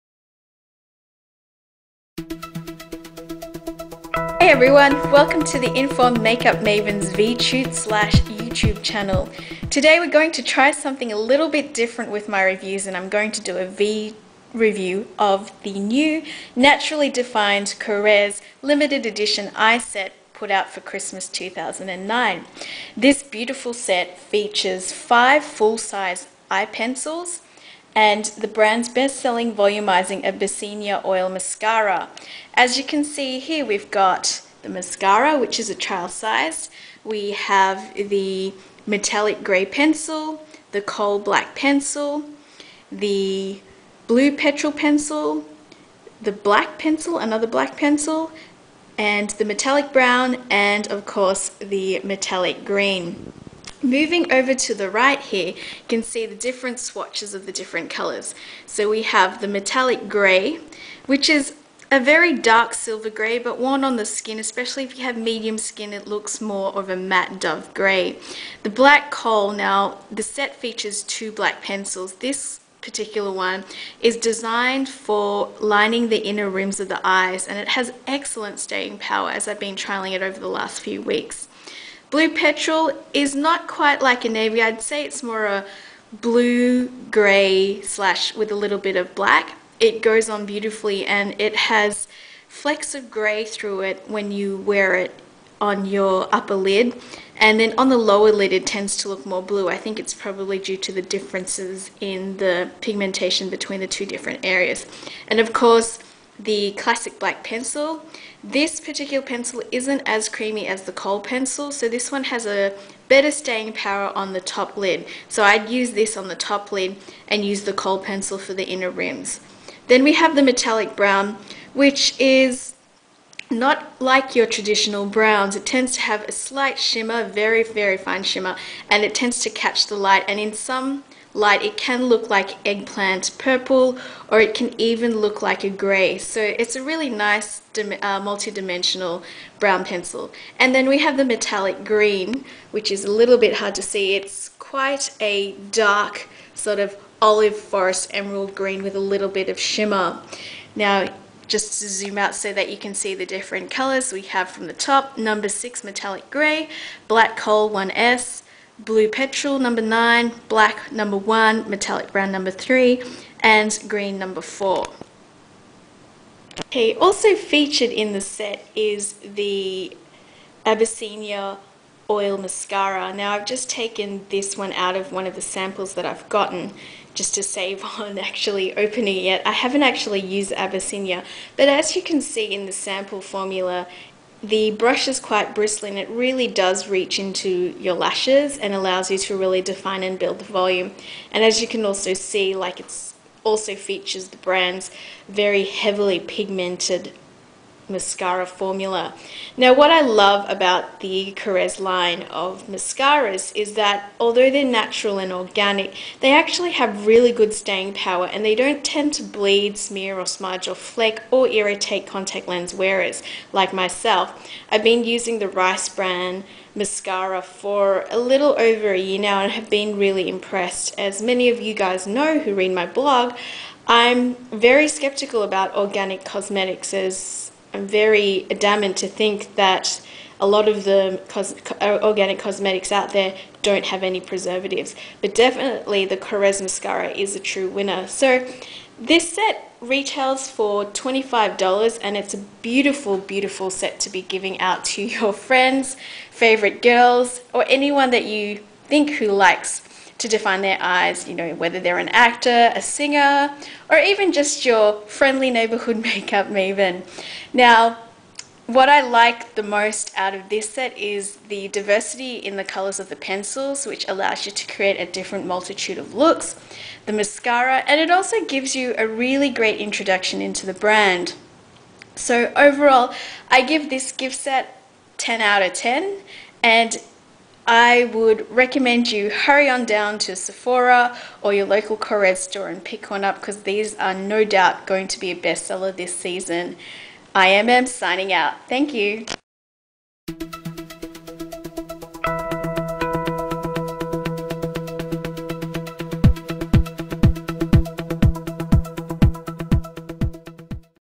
Korres Defined Eyes Review
Discover what Korres' latest holiday 2009 has to offer this season and find out IMM's verdict.